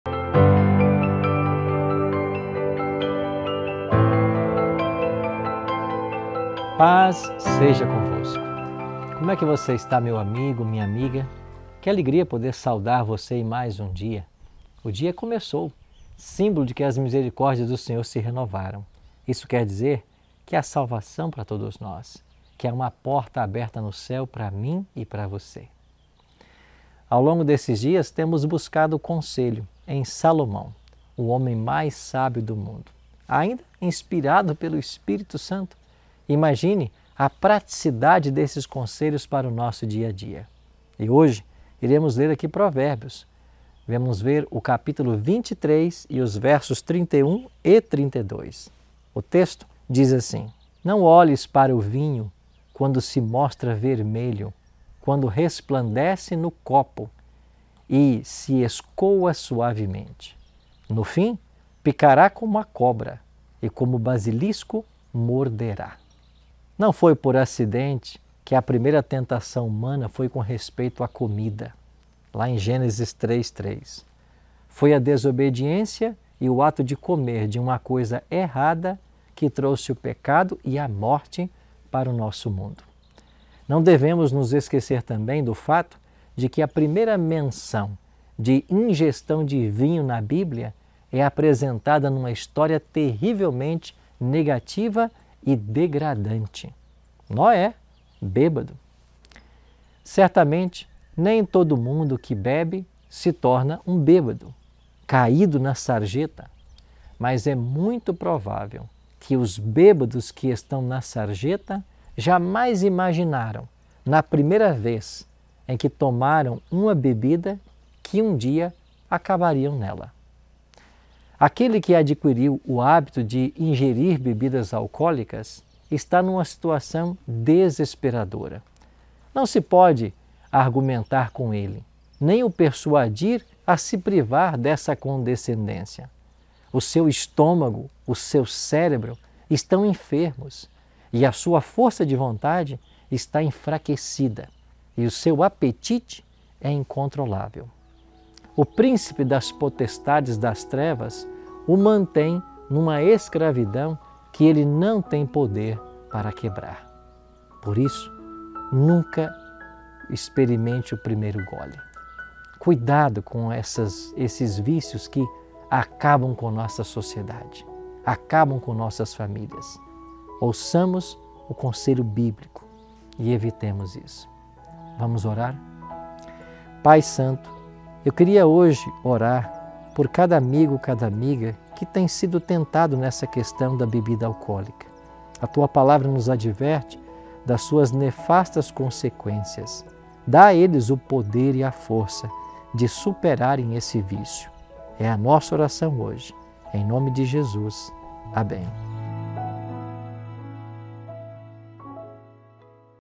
484. A BÍBLIA PROIBE BEBIDAS ALCOÓLICAS? / PRIMEIRO DEUS / PR. ARILTON
Muitos defendem usar o vinho dizendo que faz bem a saúde. O que a Bíblia fala sobre esse tema? Segue o LINK:

Este é o canal oficial do Pastor Arilton Oliveira.